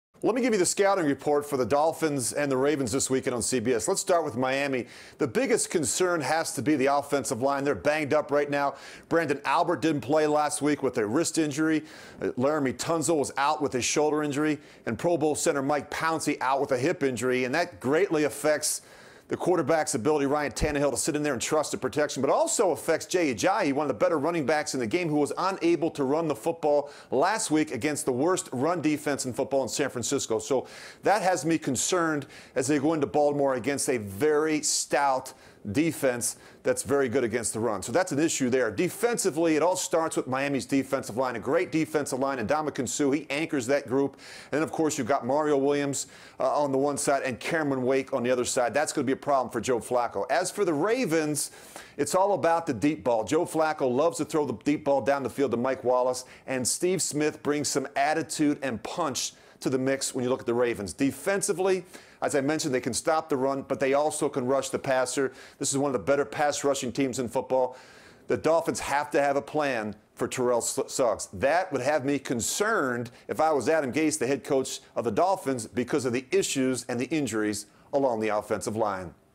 Week 13: Dolphins vs Ravens preview
Rich Gannon previews the Week 13 matchup between the Dolphins and Ravens on Sunday.